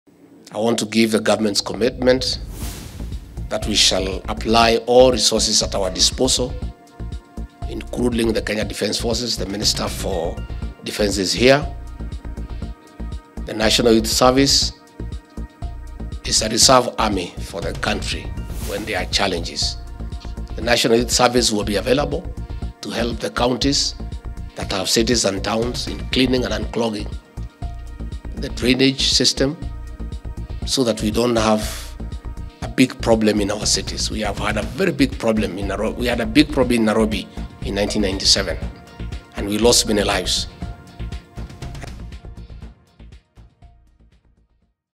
DP Rigathi convenes an El Nino response strategic meeting
Projections show that the country is likely to experience heavy rains from September 2023 to January 2024.